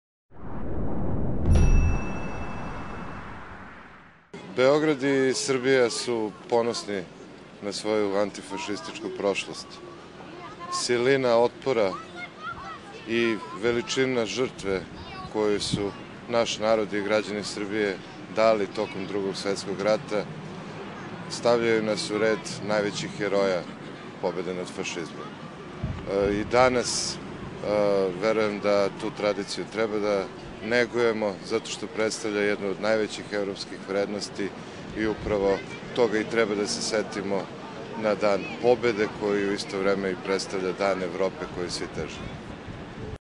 Председник Скупштине града Београда Александар Антић
Београд и Србија поносни су на своју антифашистичку прошлост. Силина отпора и јачина жртве које су наш народ и грађани Србије дали током Другог светског рата стављају нас у ред највећих хероја борбе над фашизмом, рекао је Антић након полагања венаца.